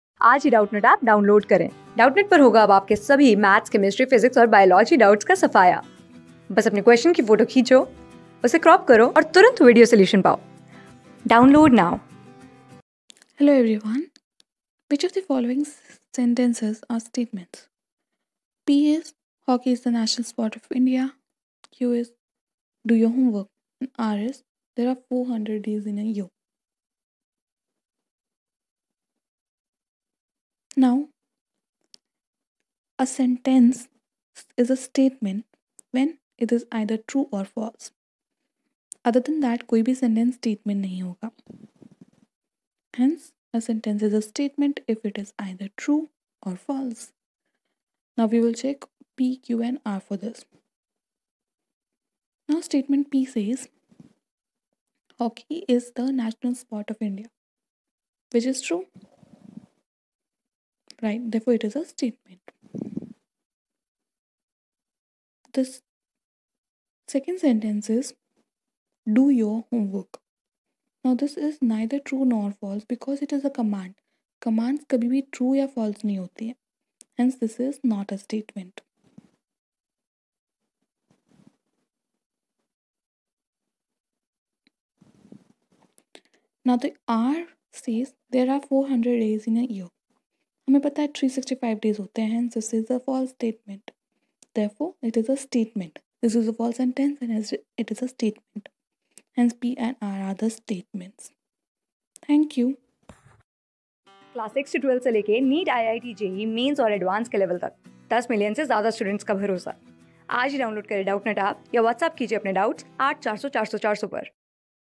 Which of the following sentences are statements p : Hockey is the national sport of Indiaq· : D...
Which of the following sentences are statements p : Hockey is the national sport of Indiaq· : Do your homework r : There are 400 days in a year
Class: 12
Subject: MATHS
Chapter: MATHEMATICAL REASONING
Board:IIT JEE

👉 Have Any Query? Ask Us.